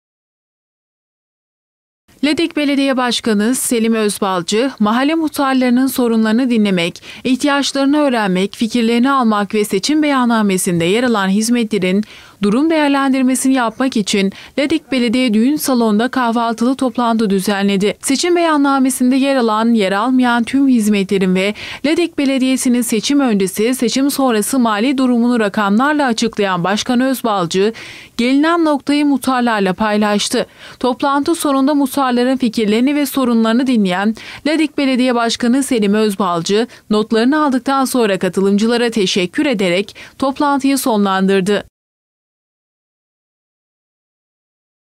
BAŞKAN ÖZBALCI MUHTARLARLA BULUŞTU
Ladik Belediye Başkanı Selim Özbalcı, daire amirleri ve esnaflarla yapmış olduğu durum değerlendirme toplantısını bu kez muhtarlarla gerçekleştirdi.